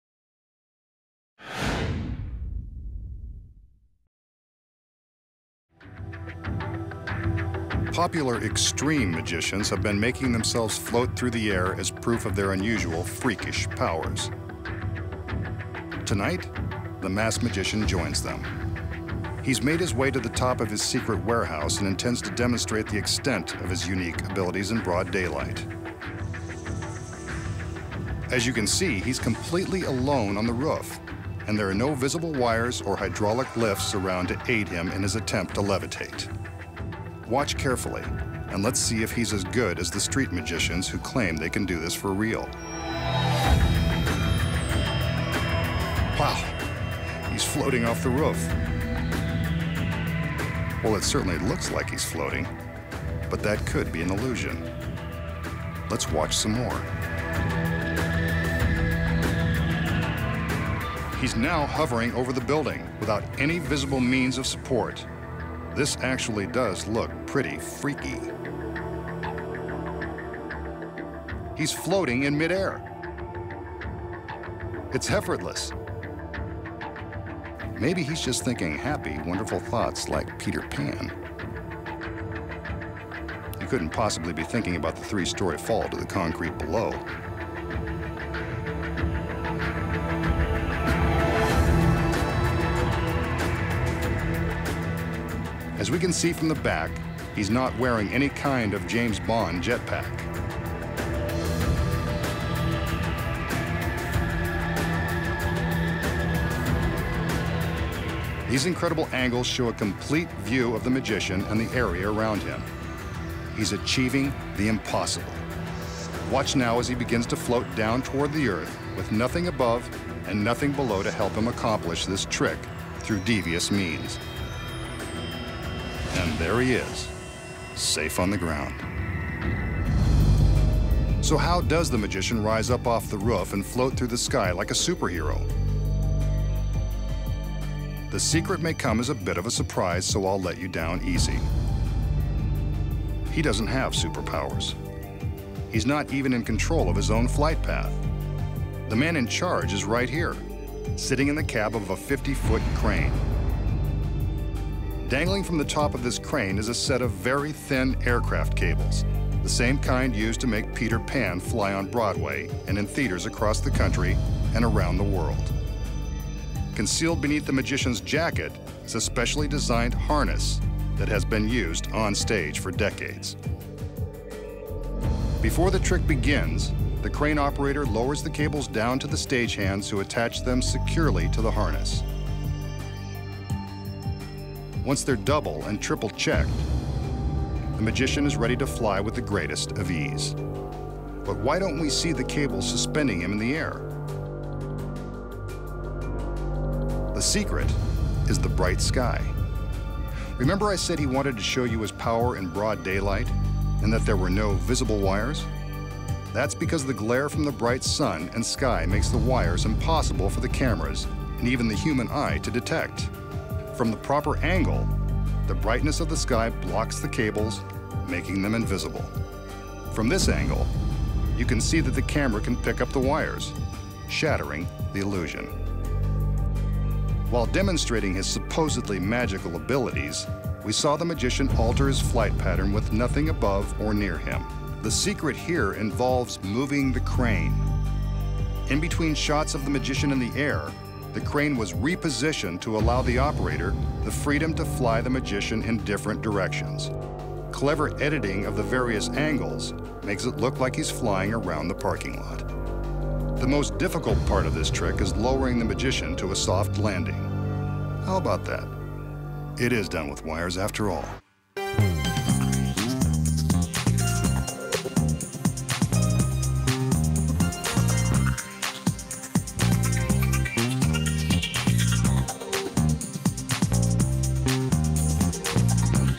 MAGICIAN PERFORMS MIND-BLOWING LEVITATION!
The Masked Magician levitates and flies over a three story building in broad daylight!  How does he do it?  Watch when he reveals the closely-guarded secret!